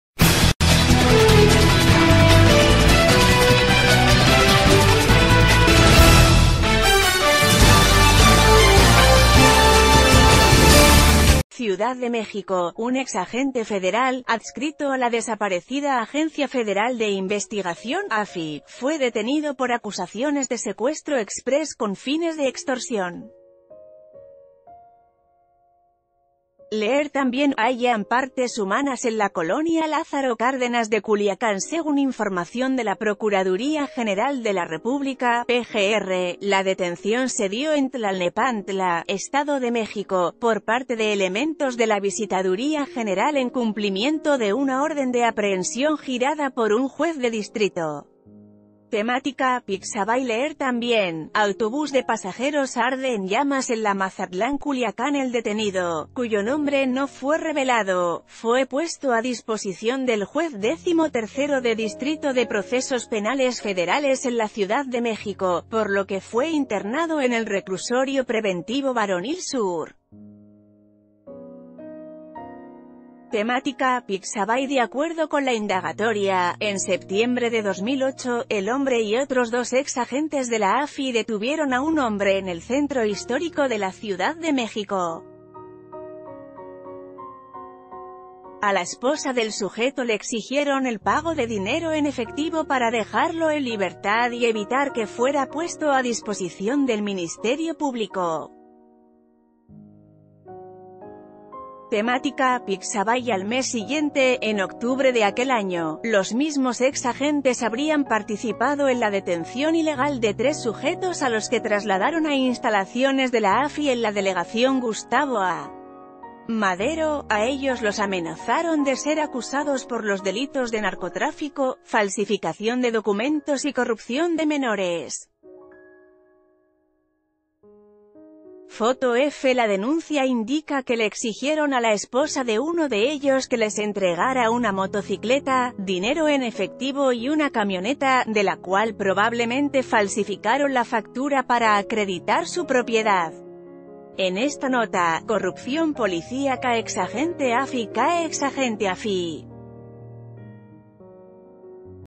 Cae exagente de la AFI por secuestro exprés
Ciudad de México.- Un ex agente federal, adscrito a la desaparecida Agencia Federal de Investigación (AFI), fue detenido por acusaciones de secuestro exprés con fines de extorsión. Leer tambien: Hallan partes humanas en la colonia Lázaro Cárdenas de CuliacánSegún información de la Procuraduría Gener...